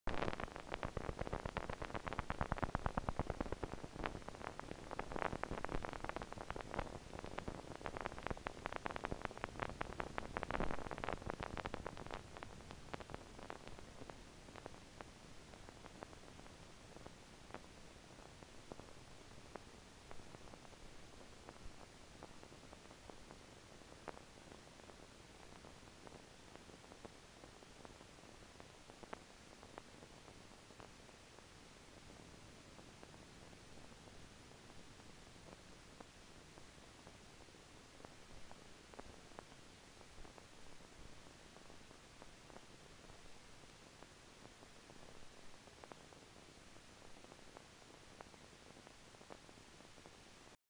Pathologic Spontainious Activity after compleat axonal damage
Pathological Spontaneous Activity (PSA) after complete axonal damage caused by a temporal bone fracture six weeks ago.